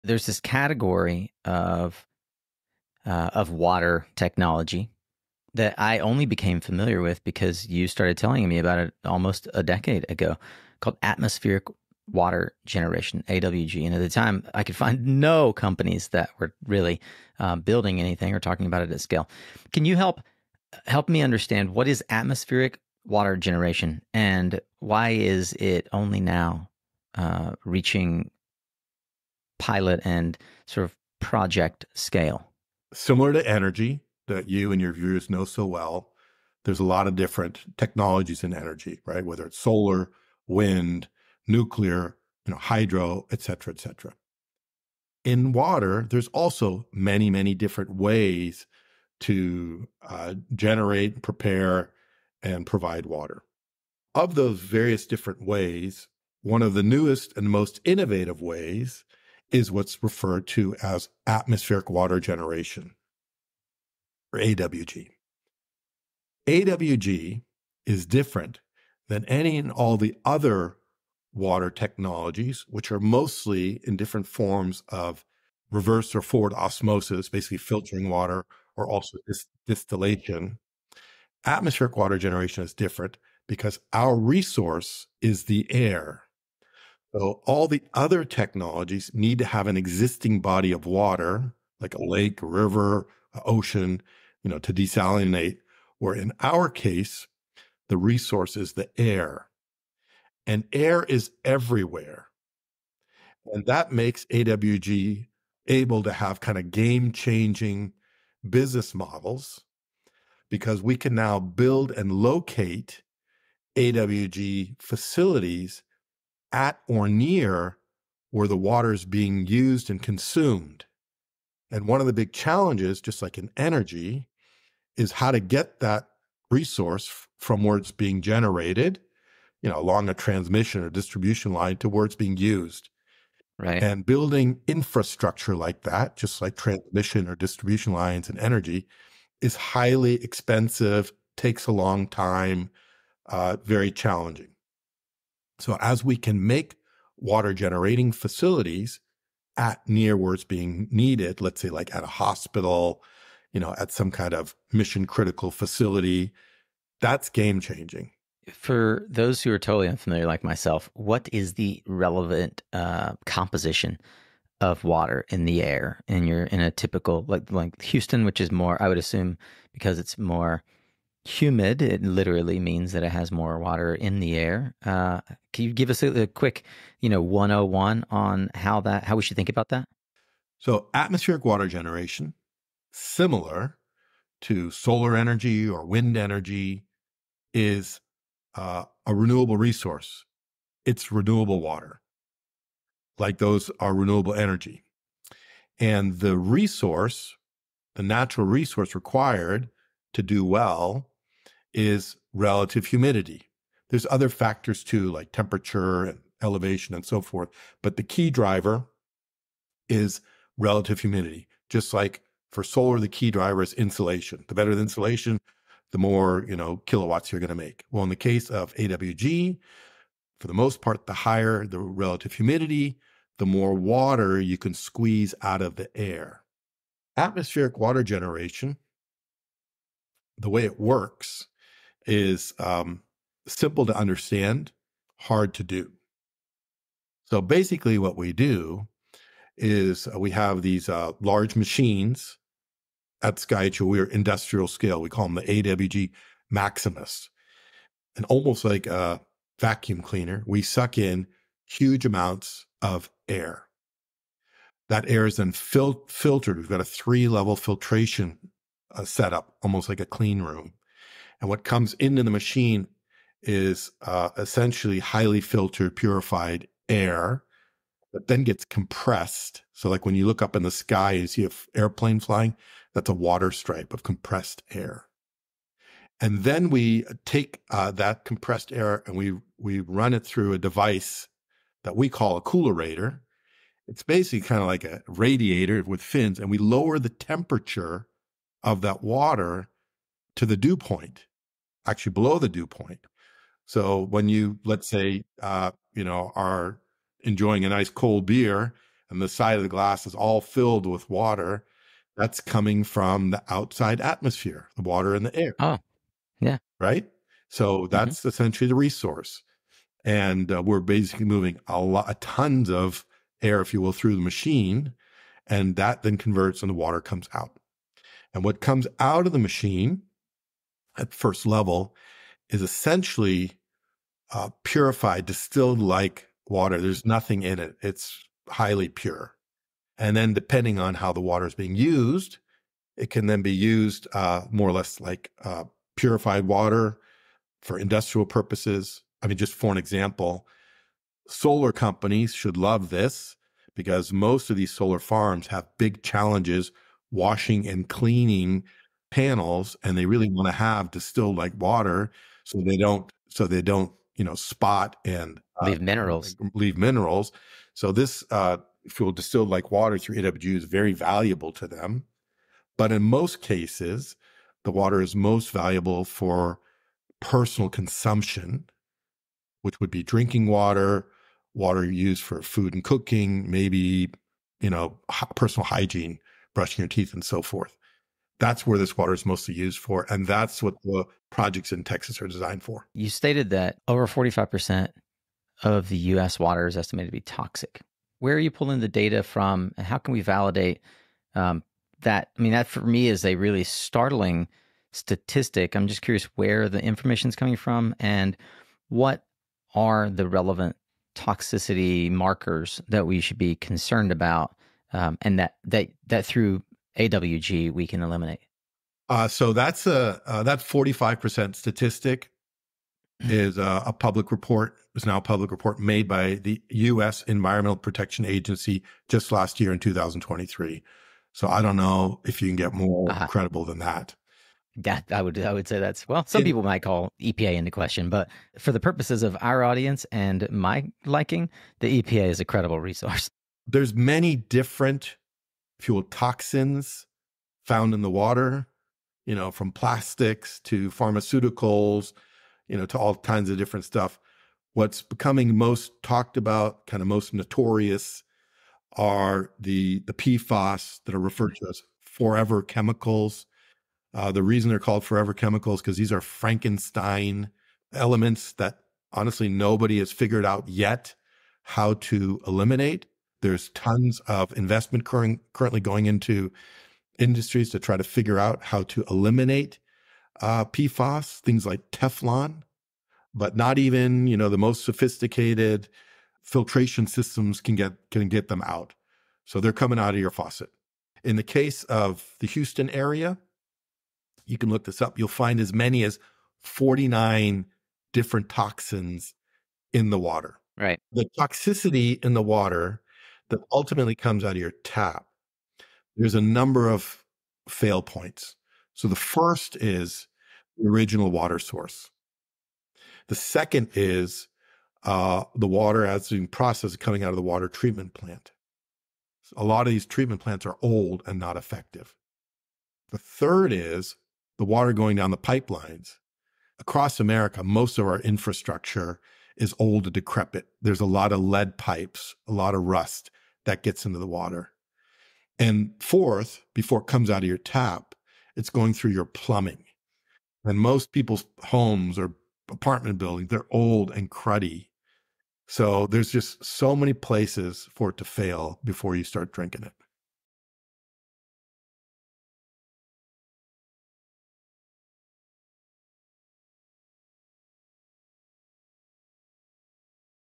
What is Atmospheric Water Generation (AWG)?
Turning Air into Water: Could this be the future of clean water?

Alexander von Welczeck, CEO of SkyH2O, introduces the technology of Atmospheric Water Generation (AWG).  AWG taps into air humidity to produce purified water, offering a groundbreaking solution to water scarcity. With potential applications ranging from industrial use to personal consumption, this technology could revolutionize how we access clean water.

00:00 What is Atmospheric Water Generation?
02:38 Is Atmospheric Water Renewable?
6:23 Tap Water Toxicity